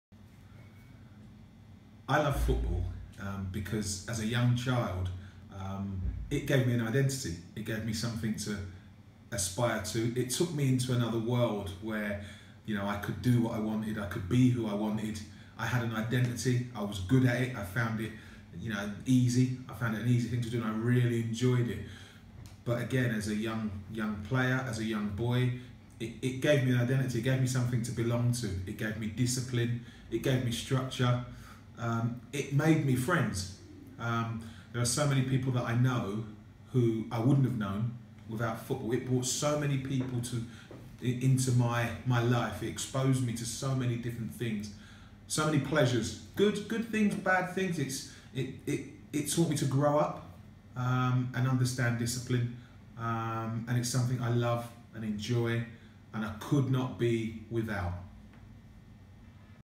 'Get Into Football Media' - Paul Mortimer
Former Charlton Athletic and Aston Villa midfielder Paul Mortimer explains his passion for football.